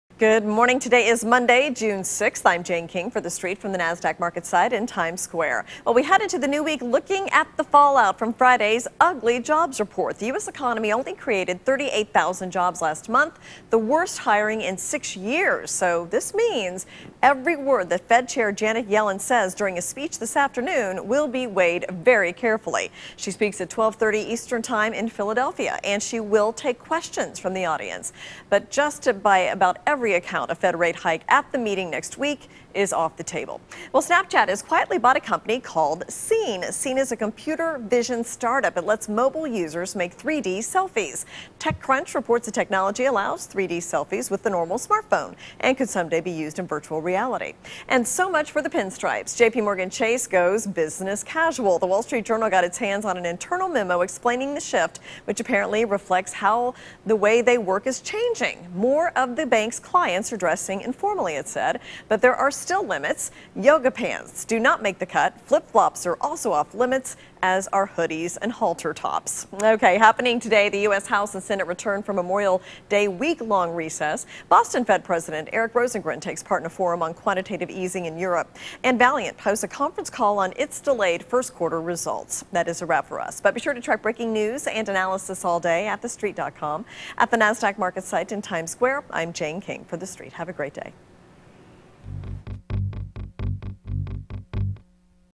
Janet Yellen Gives key Speech on Economy Today
Wall Street heads into the new week looking at the fallout from Friday's ugly jobs report. Federal Reserve Chairwoman Janet Yellen will give a speech at 12:30 ET this afternoon in Philadelphia that will be weighed very carefully. She will take questions from the audience. By just about every account, a Federal Reserve rate hike at the Fed meeting next week is off the table. Snapchat has quietly bought a company called 'Seene'. Seene is a computer vision start-up that lets mobile users make 3D selfies. Techcrunch reports the technology allows 3D selfies with a normal smartphone and could someday be used in virtual reality. JPMorgan Chase is going business casual. The Wall Street Journal reports on a memo that more of its clients are dressing informally so it will allow its bankers to dress more casual as well.